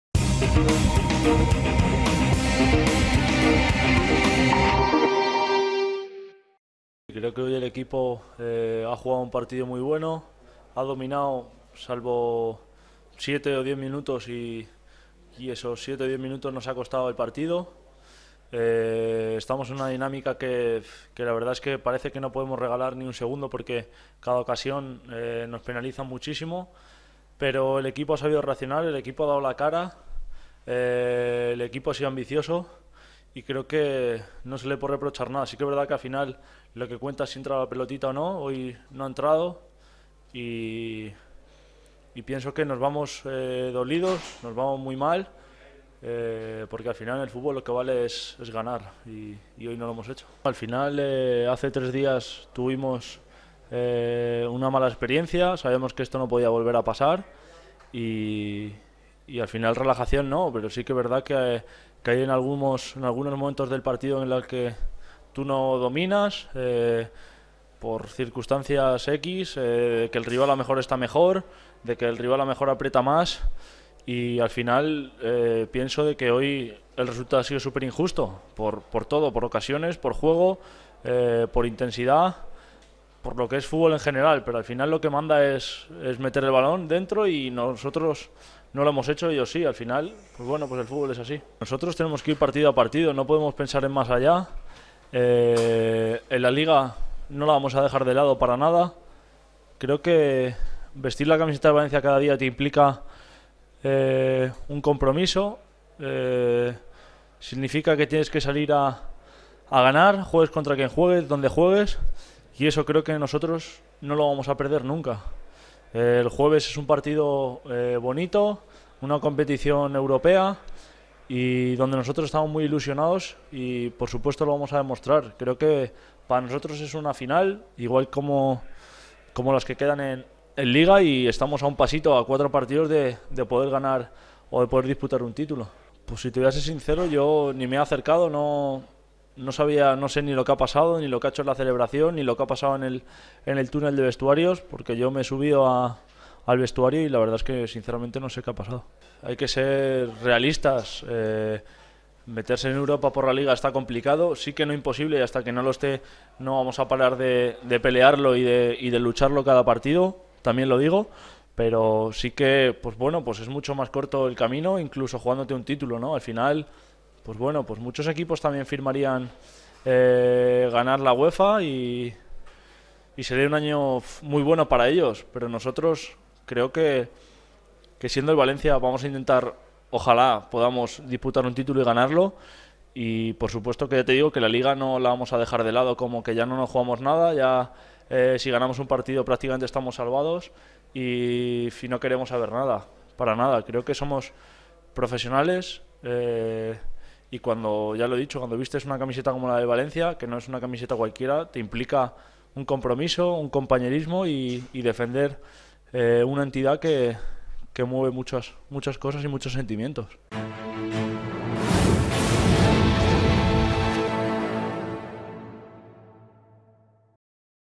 Valencia CF: Parejo 'Nos vamos dolidos'
El jugador del Valencia CF, Dani Parejo, analiza la derrota ante el Getafe CF por 1 a 3.